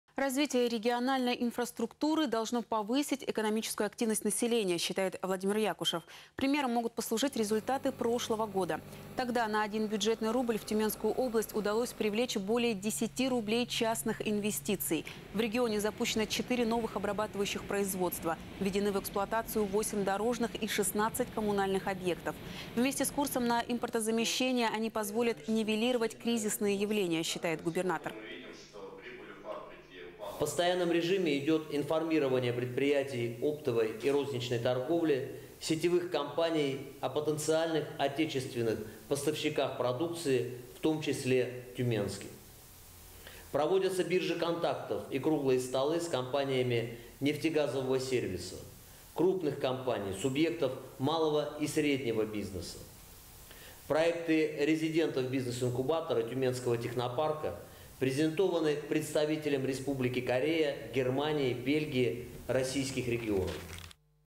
Владимир Якушев: развитие региональной инфраструктуры повысит экономическую активность населения
Развитие региональной инфраструктуры должно повысить экономическую активность населения, - считает Владимир Якушев. Примером могут послужить результаты прошлого года. Тогда на один бюджетный рубль в Тюменскую область удалось привлечь более 10 рублей частных инвестиций. В регионе запущено 4 новых обрабатывающих производства, введены в эксплуатацию 8 дорожных и 16 коммунальных объектов.